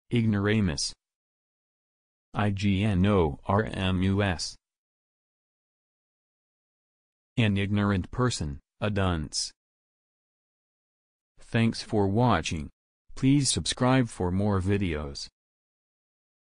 Ignoramus Meaning
Video shows what ignoramus means. An ignorant person; a dunce. Ignoramus Meaning. How to pronounce, definition audio dictionary. How to say ignoramus